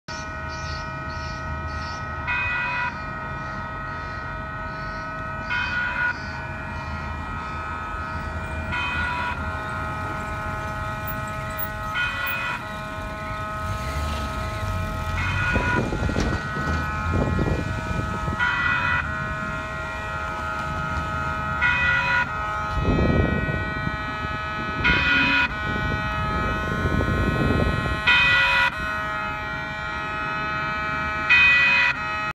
Railroad crossing malfunction at Harkins Rd, Salinas, California.
20191129 The signal wasn’t working right because a car or truck must have struck and bent the railroad crossing gate. Now the gate and the broken bell have to be fixed. The malfunction confused other drivers and caused all of them to slow down or stop.